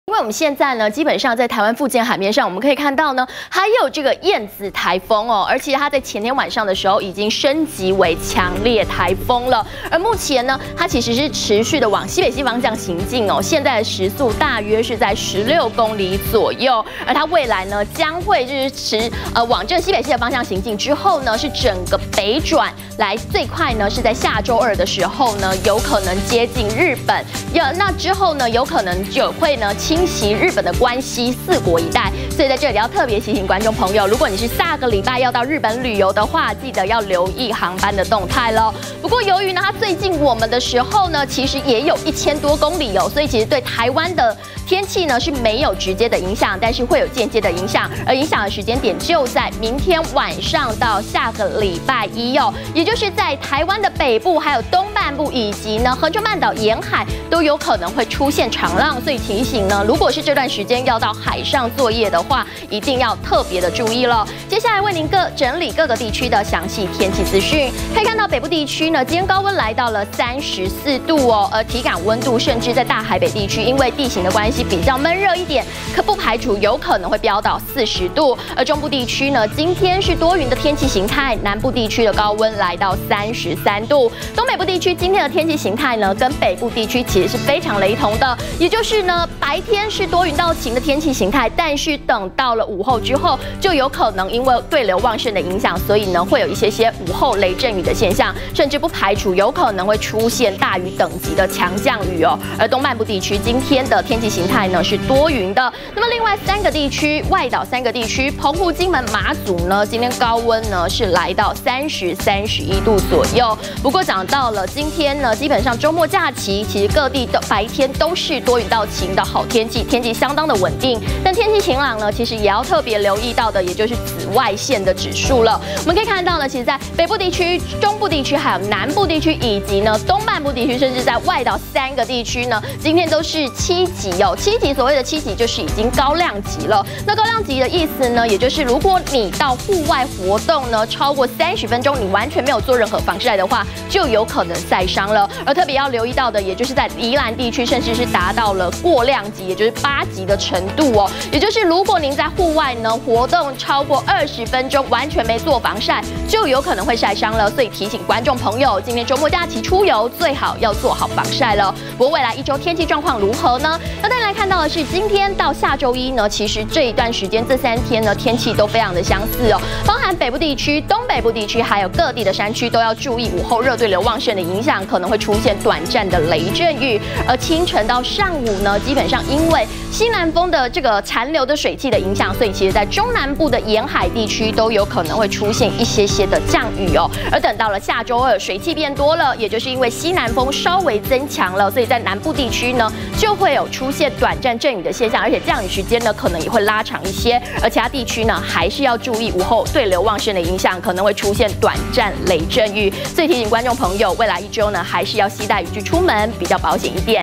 20180901中天新聞 【氣象】強颱「燕子」下週二接近日本 赴日旅遊注意
明晚至週一，北部、東部、恆春半島沿海恐現長浪，週末白天晴、午後有雨，北部高溫達34℃，北部高溫34度，大台北體感溫度40度，週末白天晴，全台紫外線指數高量級以上，中部多雲、南部高溫33度，東北部防午後雷陣雨， 下週二起西南風稍增強，南部降雨機率變高。